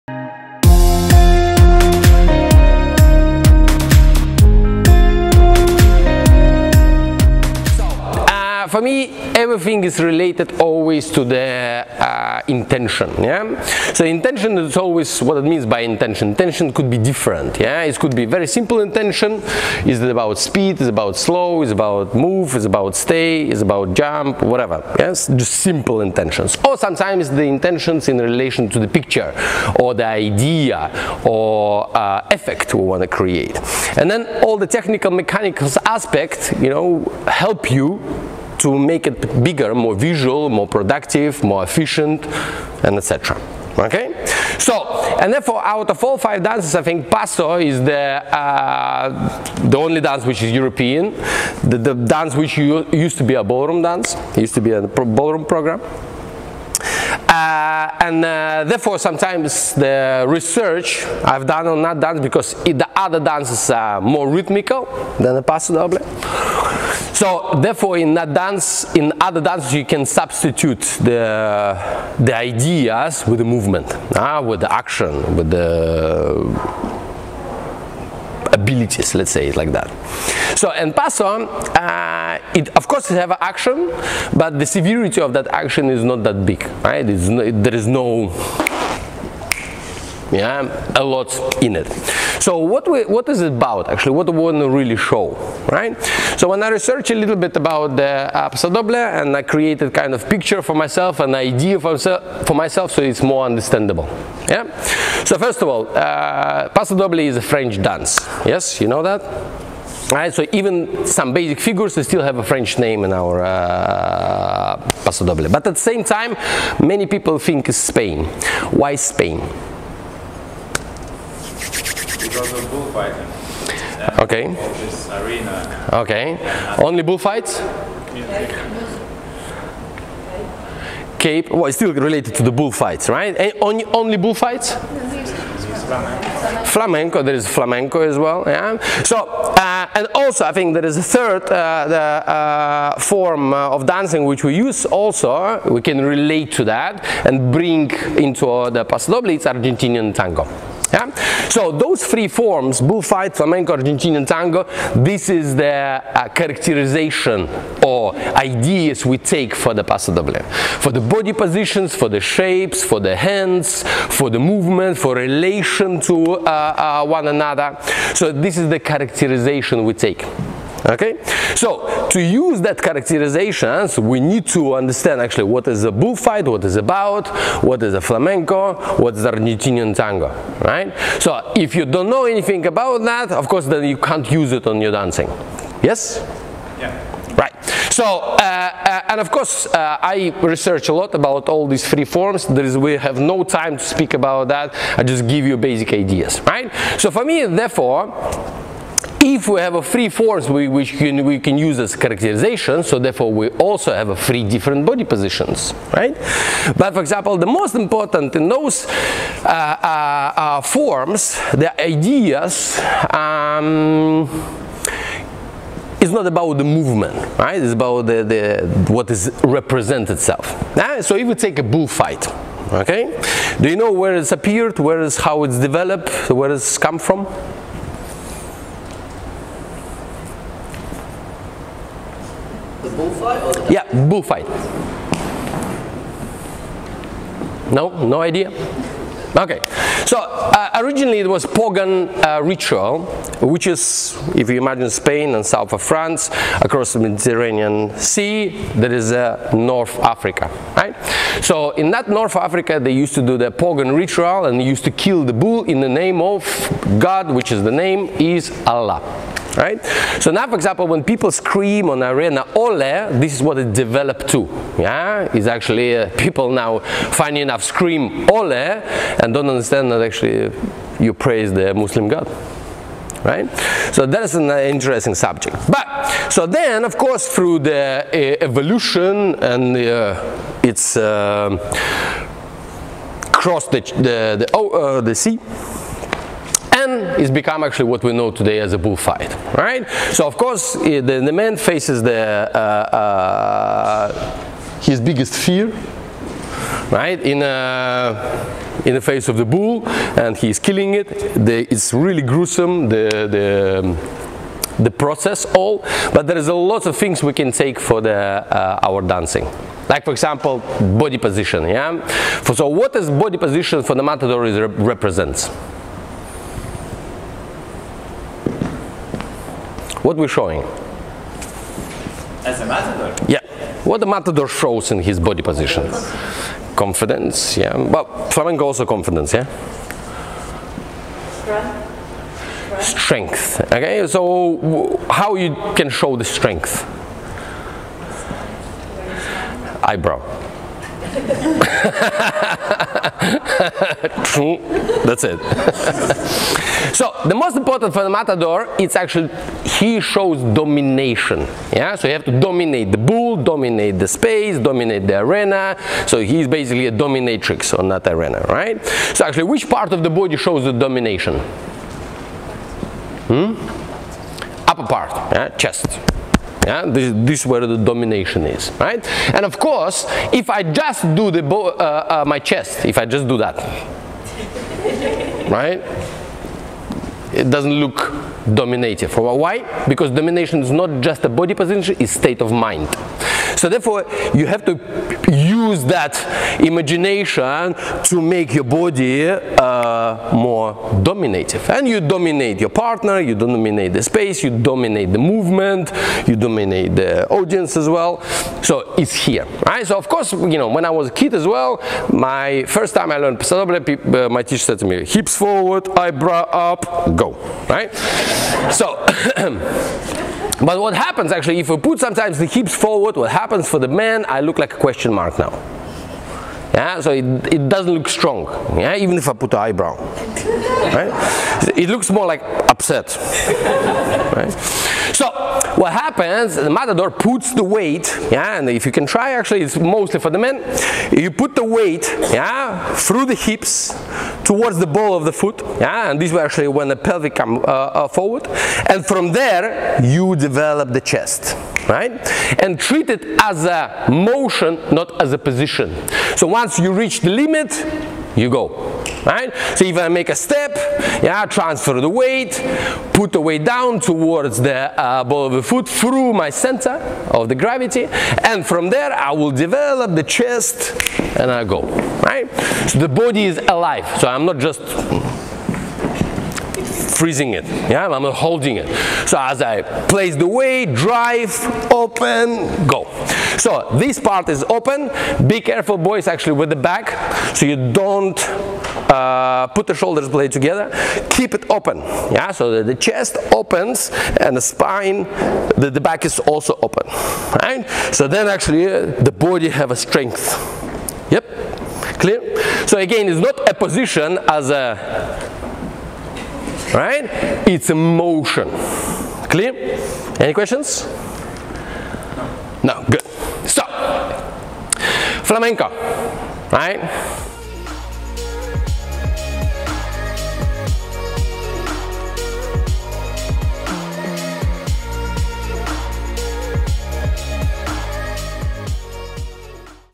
Sergey Surkov - Paso Doble (History & Technique) | Latin dance lesson pt. 1
Sergey Surkov - Paso Doble (History & Technique) | Latin dance lesson pt.1. Thank you James, Kelly & the entire Britannia Open Tiles team for choosing Dance Today to film, promote and share your amazing event with the entire world of dancesport.